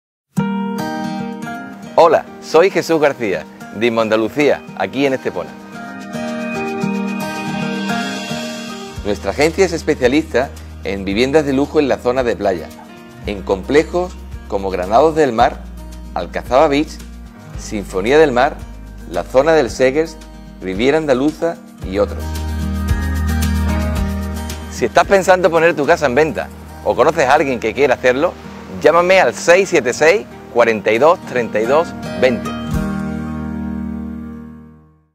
Villas, apartamentos y fincas en Estepona
Desde 1999, en Inmo-Andalucia (Estepona), venimos desarrollando nuestra actividad inmobiliaria, ayudando e informando a nuestros clientes a la hora de adquirir la propiedad de sus sueños, entre ellas lujosas villas, áticos, apartamentos y fincas en las zonas más privilegiadas de Estepona